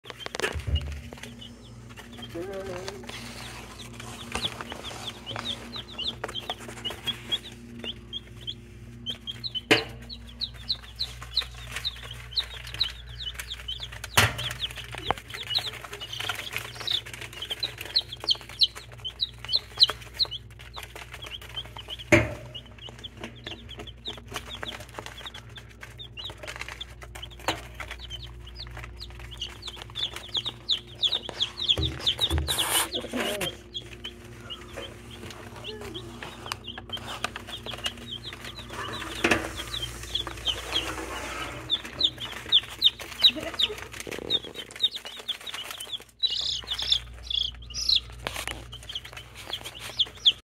2 day old baby chicks
We got our shipment of 2 day old Baby Chicks.  6 Americaunas, and 7 Speckled Sussex.  One Sussex is a rooster.